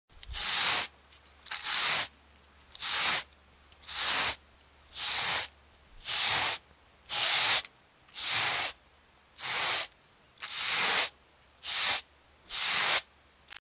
bruit de pinceau sur un mur
Gros pinceau que l'on frotte sur un mur en béton.
credit sardin